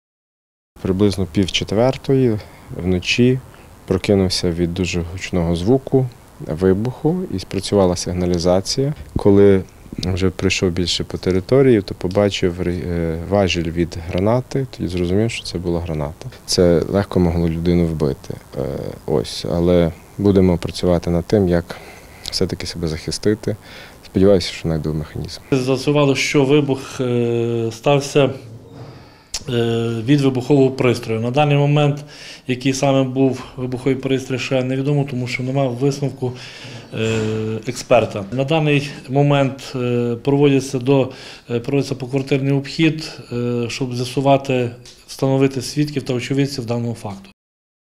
У Львові на подвір'я активіста, який бореться зі скандальною забудовою, кинули гранату
Уночі 18 серпня на подвір’я активіста, який бореться зі скандальною забудовою на Варшавській-Сосновій у Львові, кинули гранату. Вибух пролунав, коли сім'я активіста перебувала у будинку. У результаті вибуху ніхто не постраждав. Сам Руслан Козак, на подвір'я якого й кинули гранату, вважає, що це пов'язано з його громадською діяльністю і у такий спосіб його просто намагаються залякати.